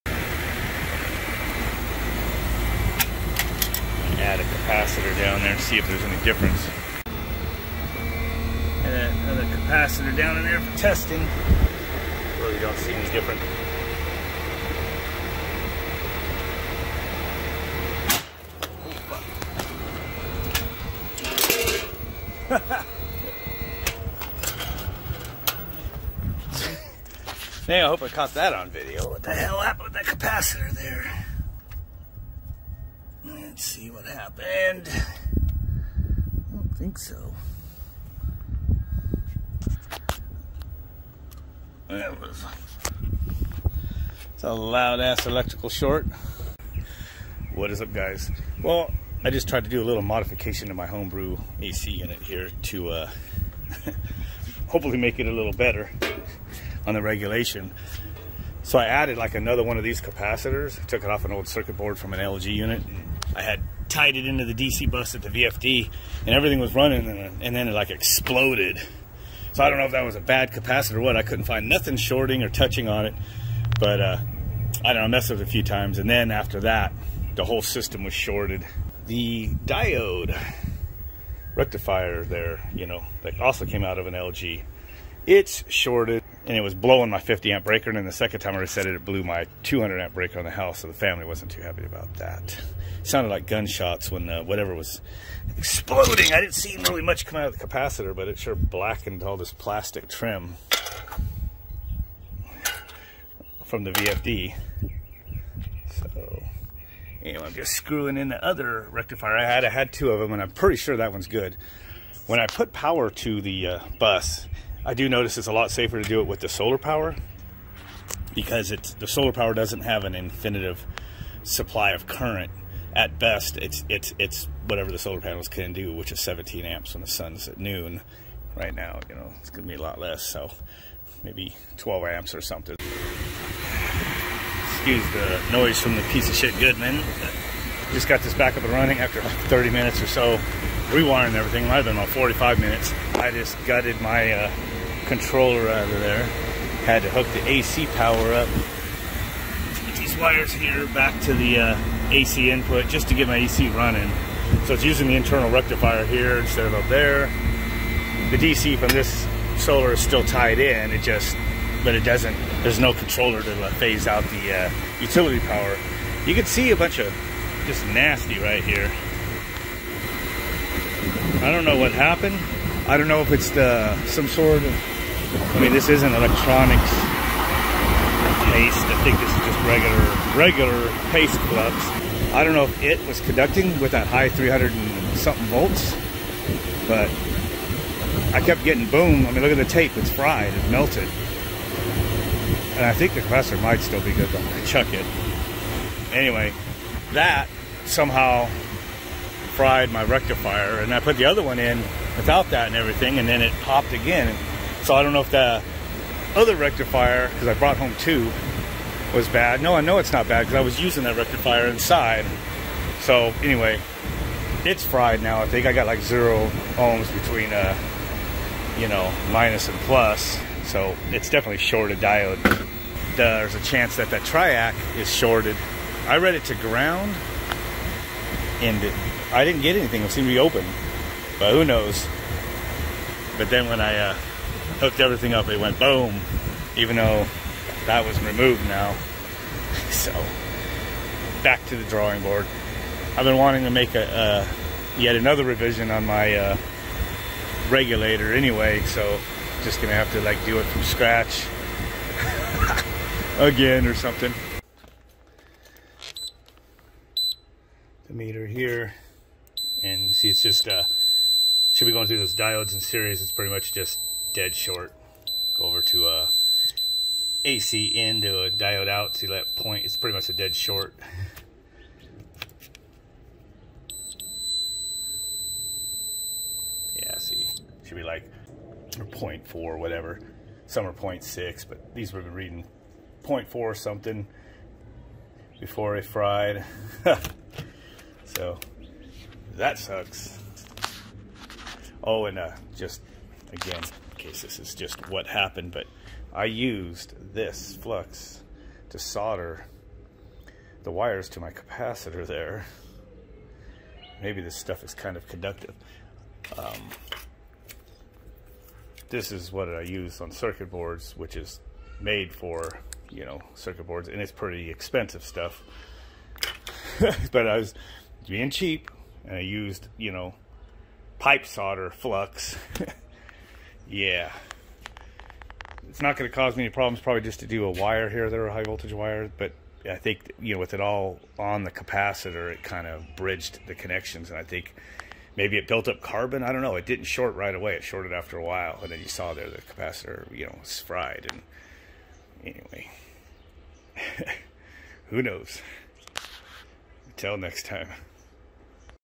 DIY Solar-Assisted heat pump goes BOOM when I soldered in another DC Bus capacitor!
Too bad. Because this concoction was actually somewhat working with the AC Utility power shutting off nearly completely with this automatic circuit.
However occasionally I was getting a fault in my VFD if I set my voltage threshhold too low. I was thinking that the DC bus may not be perfectly regulated and occasionally seeing some low voltage spike or something.
If I kept the DC BUS voltage setpoint high enough it seemed pretty solid but would draw a little bit more utility power than if I set the threshhold a bit lower but then it would glitch randomly.

So after work yesterday I got a wild hair and thought I'd solder up some leads onto a large capacitor and connect it to my VFD DC Bus.
Shortly after that BOOM!

Then off camera a BOOM again and my rectifier (and possibly my triac) shorted, causing my breaker to trip.

So to get the heat pump operating again I had to wire the AC power back to the VFD AC Power input.

I will need to continue my experiments with my utility power regulator circuit.
With the way it is wired now (both utility and solar) the best I get for solar powered is 50%/50% AC power and Solar power at high noon.